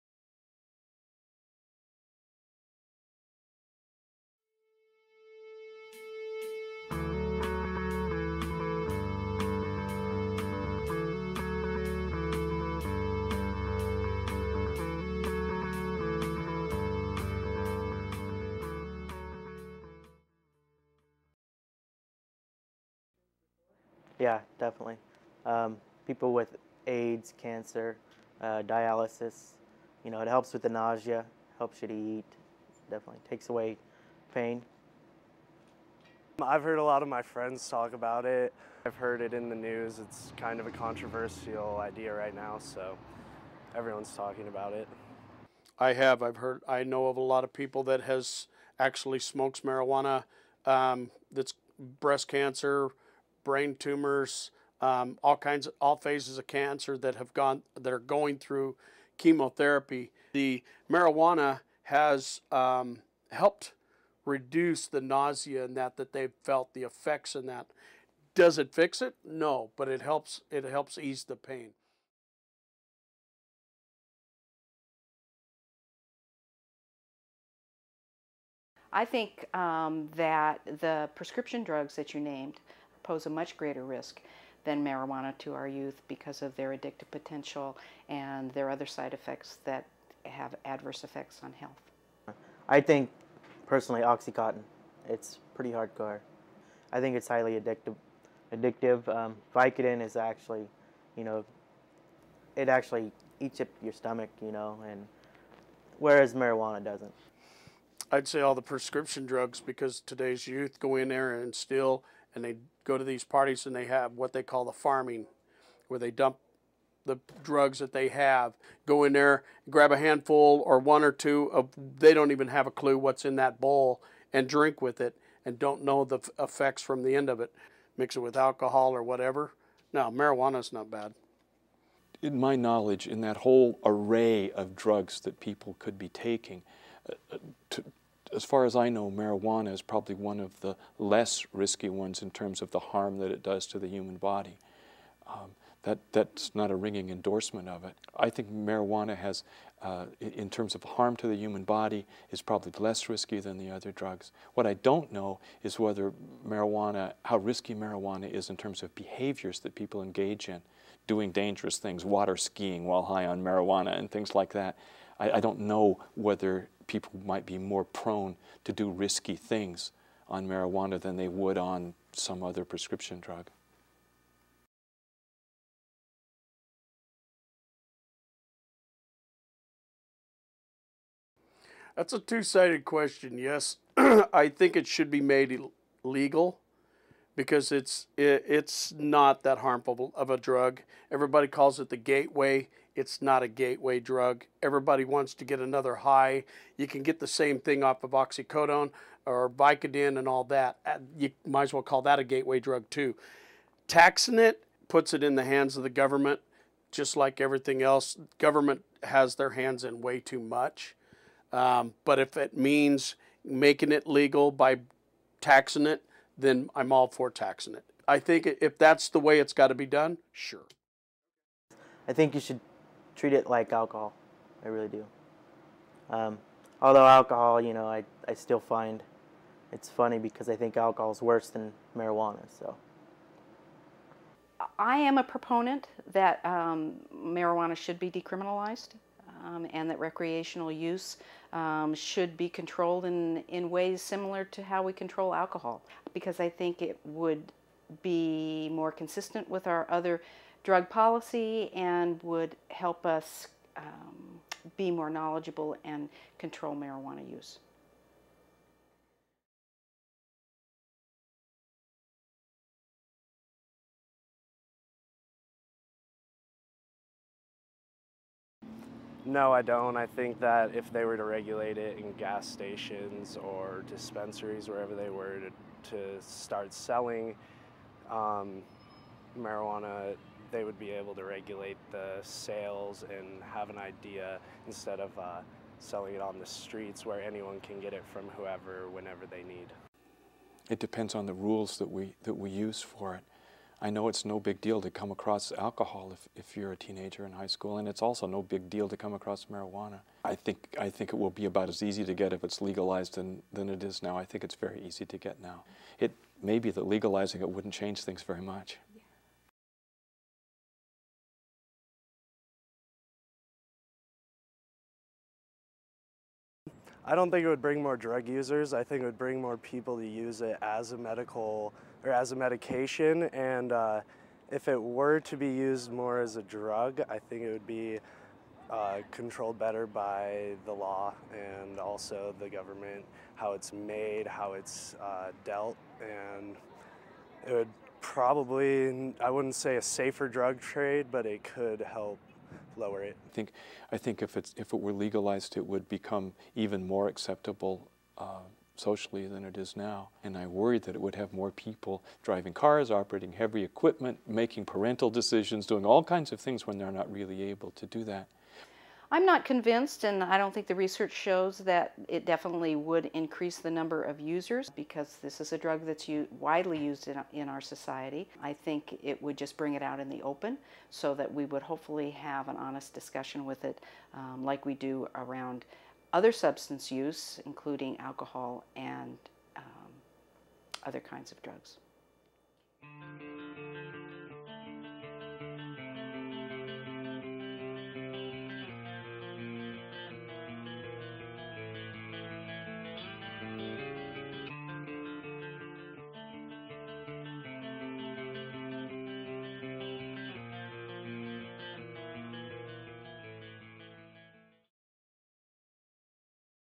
Medical Marijuana
This was created for University Television Productions at Boise State University.  This is part of a larger documentary discussing the legalization of medical marijuana in Idaho.  It was edited using Final Cut Pro.